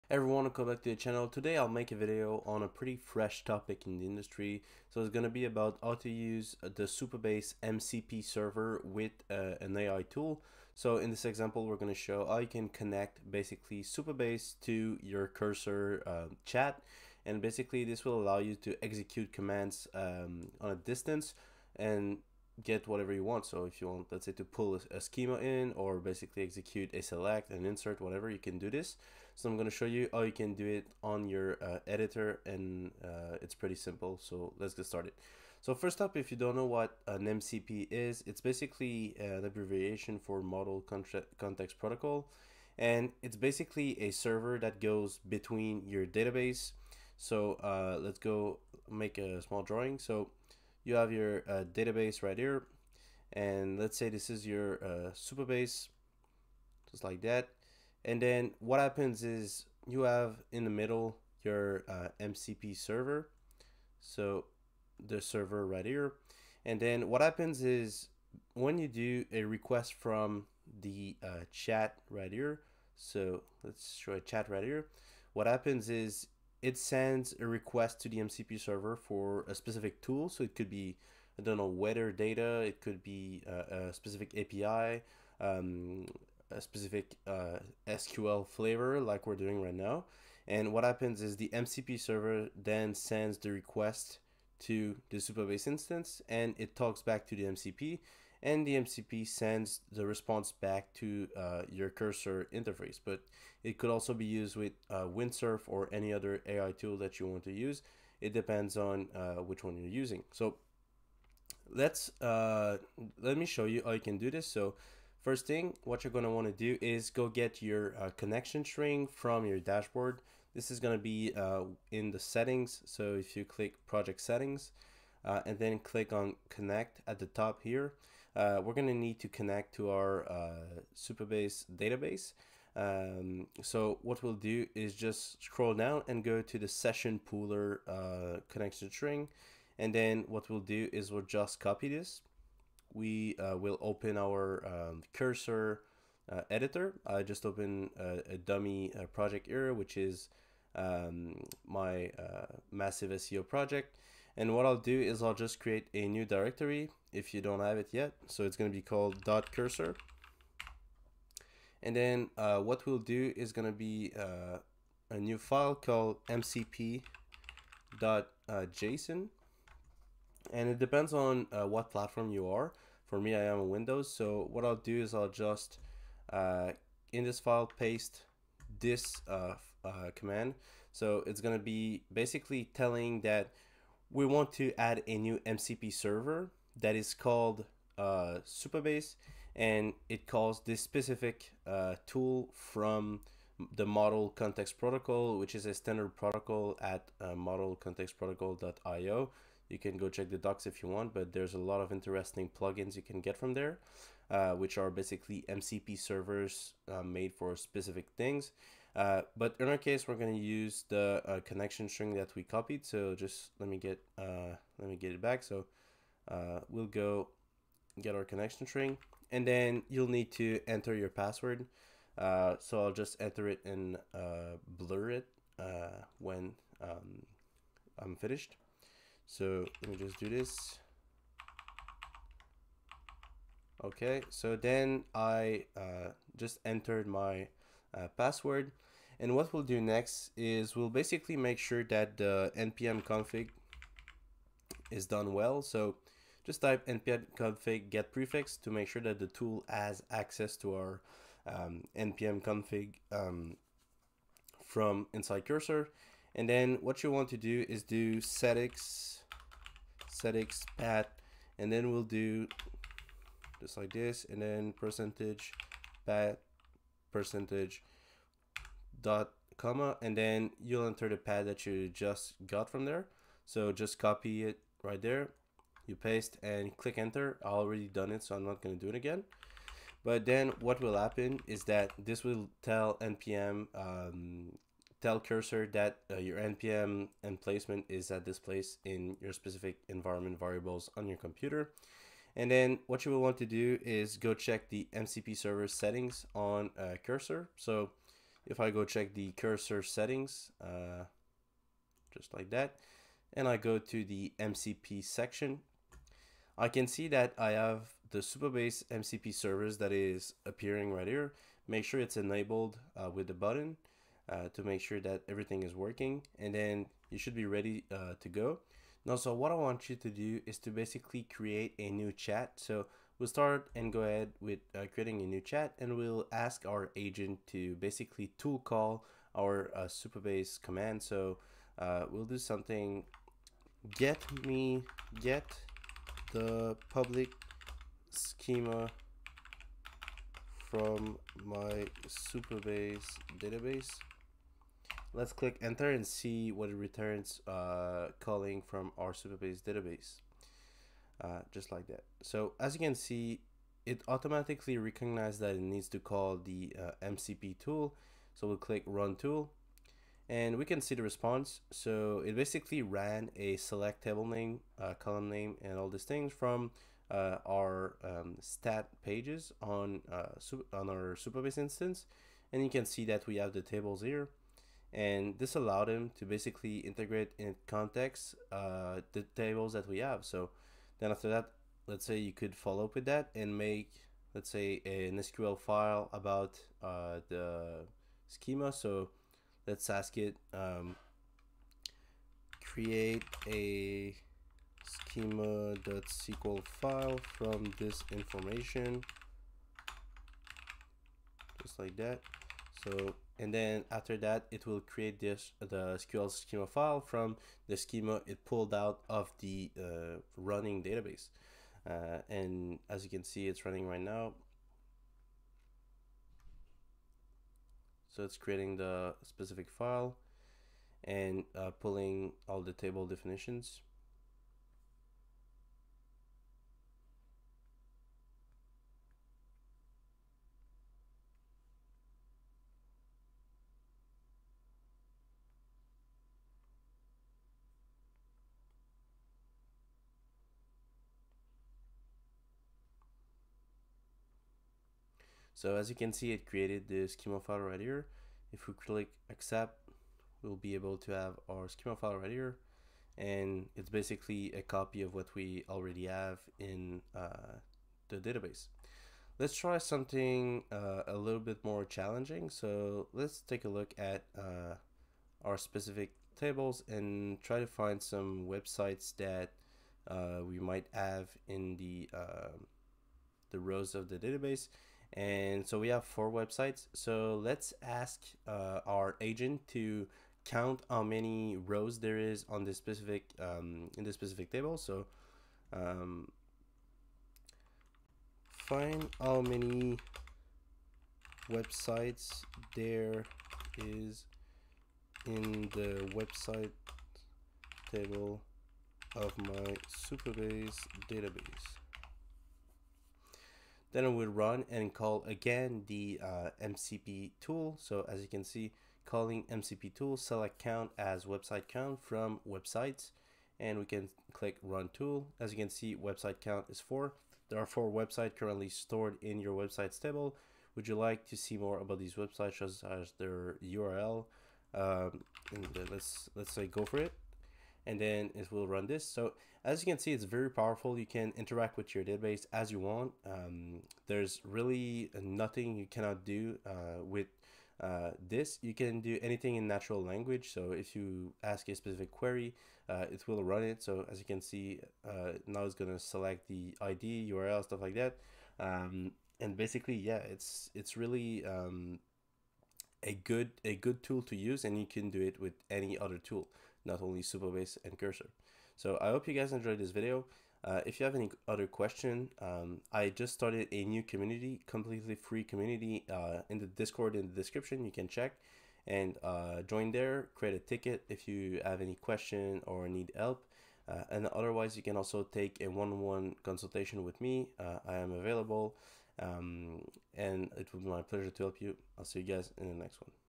How to Connect Supabase MCP Server with Cursor in 5 Minutes!
In this video I show you how to connect your Supabase database to Cursor via their MCP server (only available with the hosted version).

Timestamps:
00:00 Intro
00:35 What is MCP?
01:55 MCP Setup
05:25 Test MCP
09:24 Community and 1-1 Call